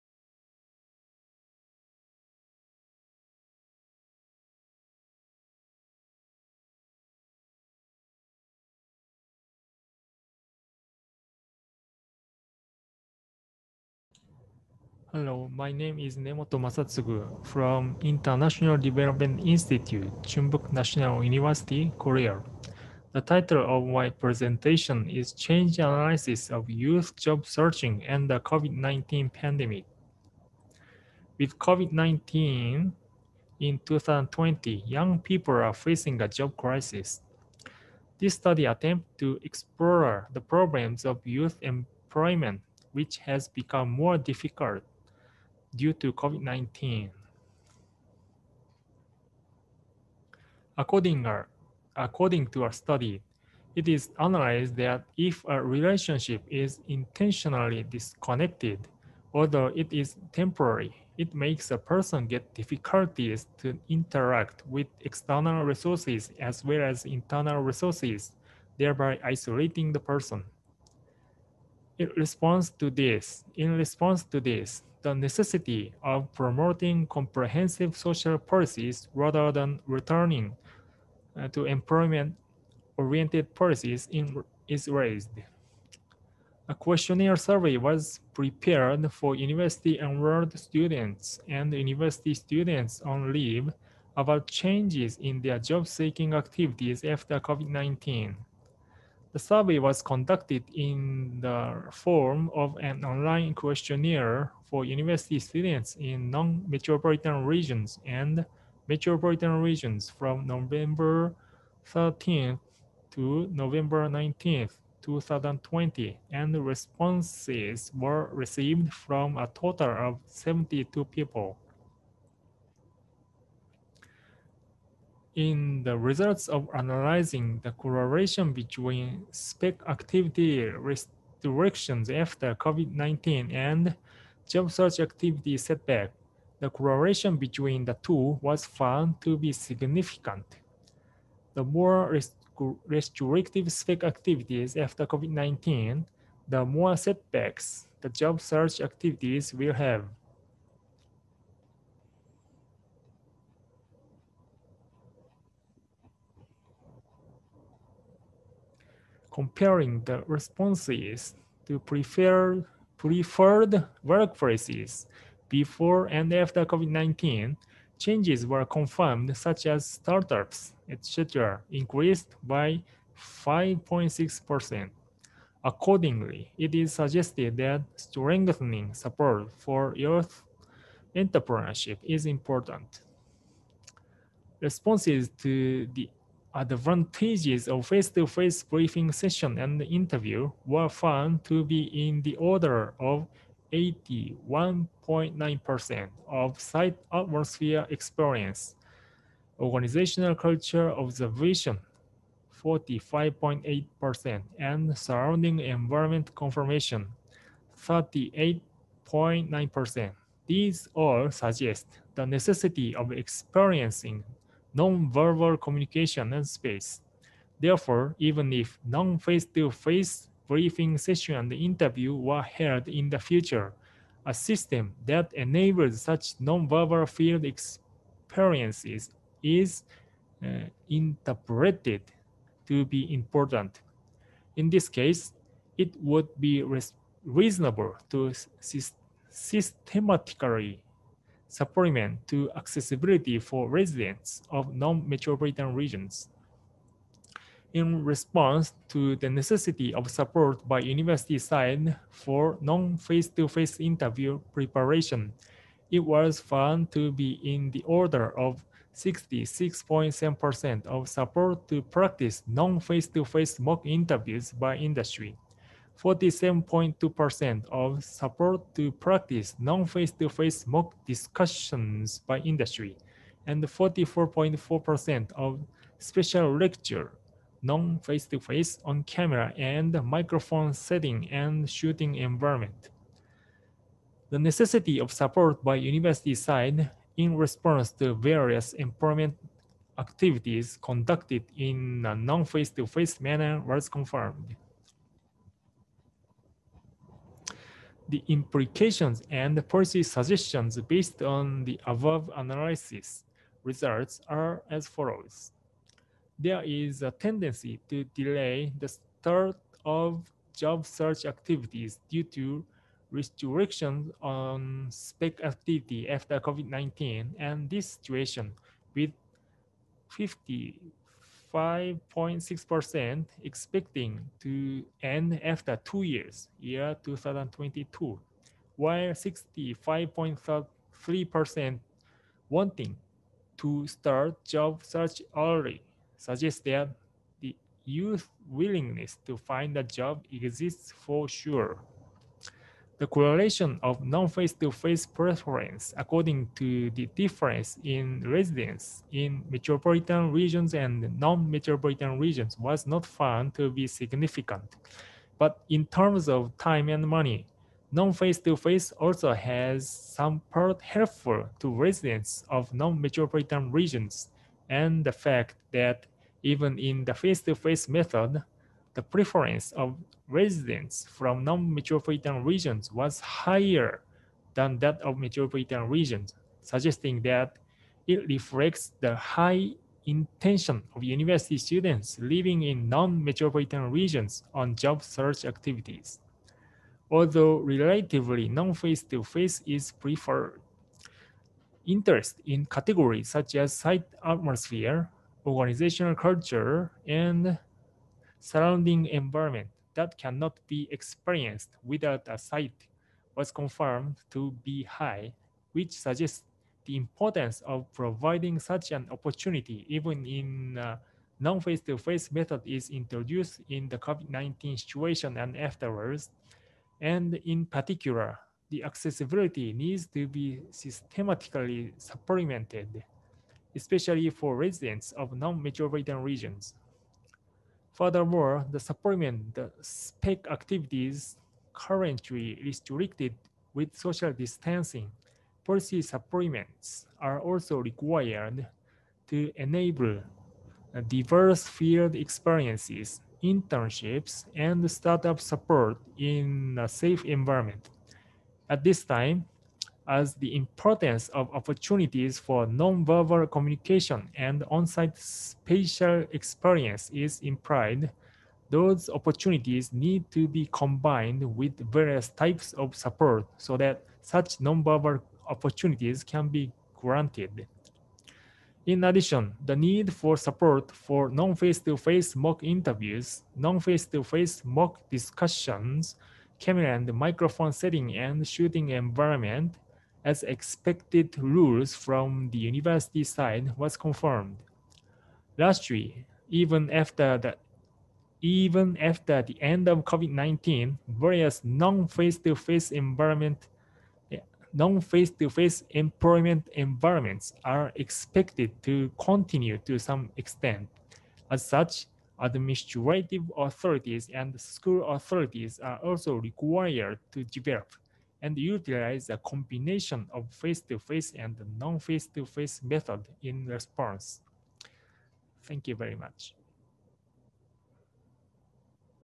[PresentationⅠ Military System, Urban Crisis, and Disaster Kit]Nemoto Masatsugu
The 21st Asian Conference on Crisisonomy & The 1st Crisisonomy Forum
The 14th International Conference on Crisis & Emergency Management
[Korea, China, Japan, U.S.A., United Kingdom, Thailand, Taiwan, Pakistan, Nepal, Mongolia, India]